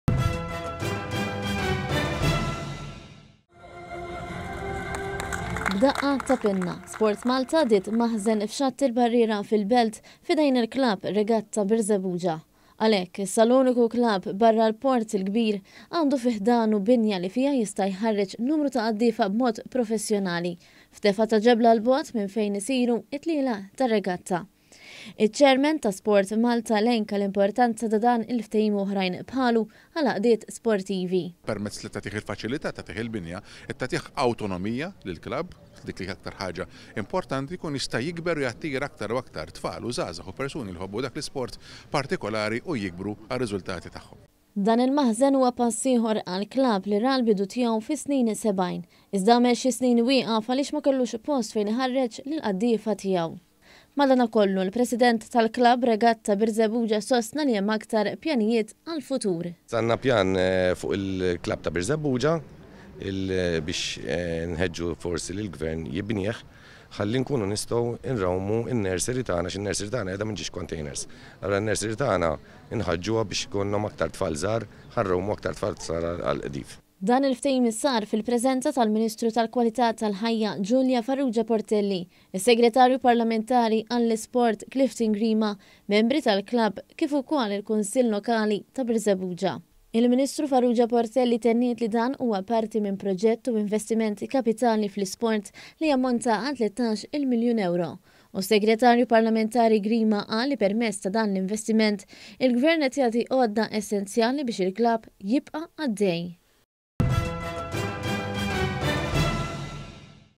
IL-GVERN JAGĦTI MAĦŻEN LILL-KLABB TAR-REGATTA TA’ BIRŻEBBUĠA
ONE.COM.MT • SportMalta llum għaddiet maħżen f’Xatt il-Barriera fil-Port il-Kbir f’idejn il-Klabb tar-Regatta ta’ Birżebbuġa.

Dan huwa fost proġetti u investiment kapitali fl-isport li jammonta għal tlettax-il miljun ewro.

KITBA: LEANNE ABELA GRECH
FILMAT: CHRIS BORG

B’daqqa ta’ pinna SportMalta għaddiet maħżen f’Xatt il-Barriera fil-Belt f’idejn il-Klabb Regatta Birżebbuġa. Għalhekk issa l-unika klabb barra l-Port il-Kbir għandu fi ħdanu binja li fiha jista’ jħarreġ numru ta’ qaddiefa b’mod professjonali f’tefgħa ta’ ġebla ‘l bogħod minn fejn isiru t-tlielaq tar-regatta.

Iċ-Chairman ta’ SportMalta elenka l-importanza ta’ dan il-ftehim u oħrajn bħalu għal għaqdiet sportivi.

LUCIANO BUSUTTIL
CHAIRMAN TA’ SPORTMALTA
“Permezz li tatih il-faċilità, tatih il-binja, qed tatih awtonomija lill-klabb, dik hija l-aktar ħaġa importanti, jkun jista’ jikber u jattira aktar tfal u żgħażagħ u persuni li jħobbu dak l-isport partikolari u jikbru għar-riżultati tagħhom.”

Dan il-maħżen huwa pass ieħor għall-klabb li ra l-bidu tiegħu fis-snin sebgħin, iżda għamel xi snin wieqaf għaliex ma kellux post fejn iħarreġ lill-qaddiefa tiegħu.

Madanakollu, il-President tal-Klabb Regatta Birżebbuġa sostna li hemm aktar pjanijiet għall-futur.

IAN FARRUGIA
PRESIDENT TA’ KLABB REGATTA BIRŻEBBUĠA
“Għandna pjan fuq il-klabb ta’ Birżebbuġa biex inħeġġu lill-Gvern jibnih ħalli nkunu nistgħu nrawmu in-nursery tagħna, għaliex in-nursery tagħna qiegħda f’żewġ containers. Allura n-nursery tagħna inħeġġuha biex ikolna aktar tfal żgħar ħa nrawmu aktar tfal għall-qdif.”

Dan il-ftehim sar fil-preżenza tal-Ministru tal-Kwalità tal-Ħajja Julia Farrugia Portelli, is-Segretarju Parlamentari għall-Isport, ir-Rikreazzjoni u l-Volontarjat Clifton Grima, membri tal-klabb kif ukoll il-Kunsill Lokali ta’ Birżebbuġa.

Il-Ministru Farrugia Portelli tenniet li dan huwa parti minn proġetti u investiment kapitali fl-isport li jammonta għal 13-il miljun ewro u s-Segretarju Parlamentari Grima qal li permezz ta’ dan l-investiment il-Gvern qed jagħti għodda essenzjali biex il-klabb jibqa’ għaddej.